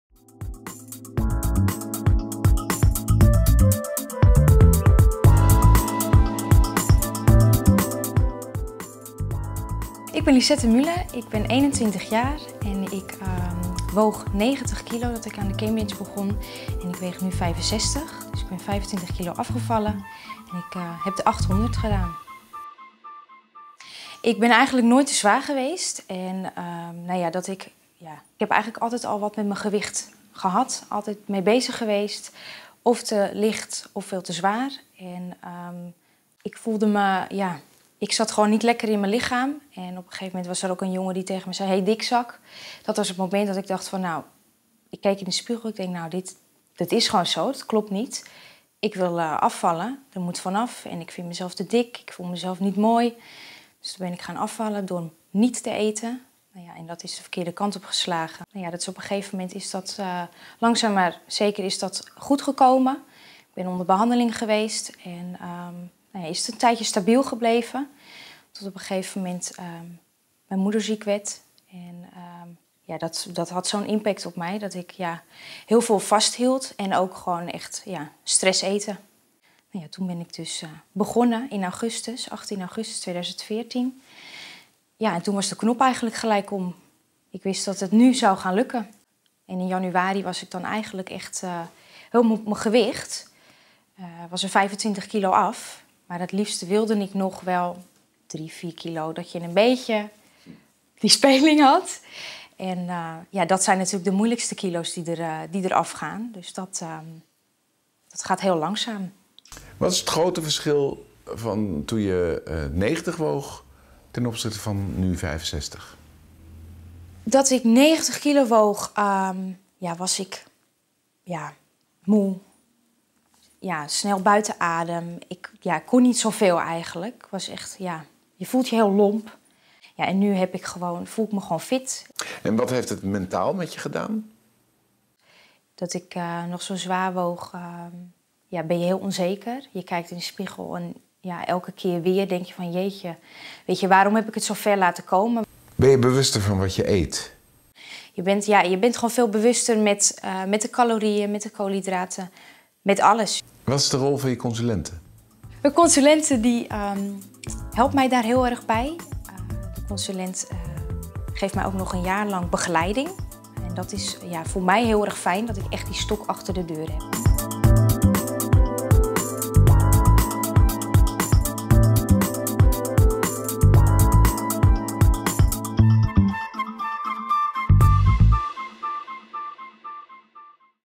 Lisette Muller, Finalist Afslanker van het Jaar 2015 Cambridge dieet 800
Jaarlijks vindt de Afslankers en Gewichtsbehouder van het Jaar verkiezing plaats. Deze verkiezing kent drie categorieën: Afslanker van het Jaar Cambridge dieet 500, Afslanker van het Jaar Cambridge dieet 800 en Gewichtsbehouder van het Jaar.